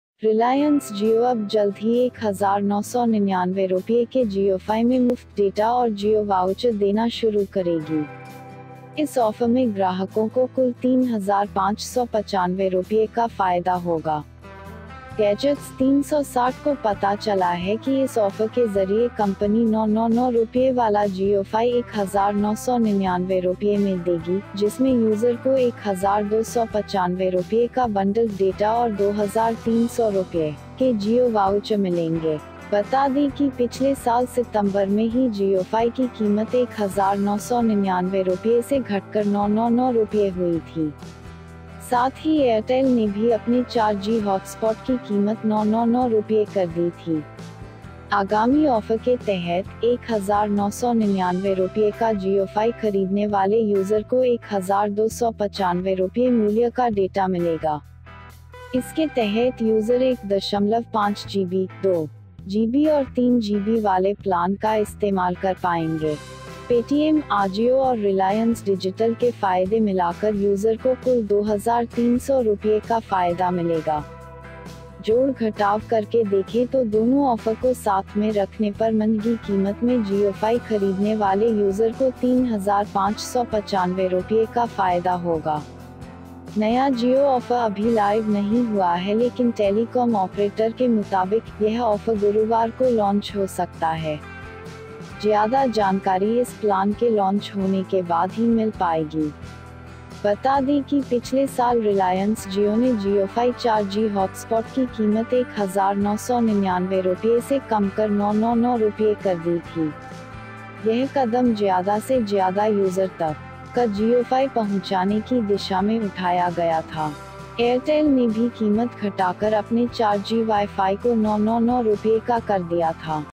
जियोफाई खरीदें फायदा ही फायदा 1,999 रुपये में, होगा 3,595 रुपये का फायदा II Gadget Gyan
Asal News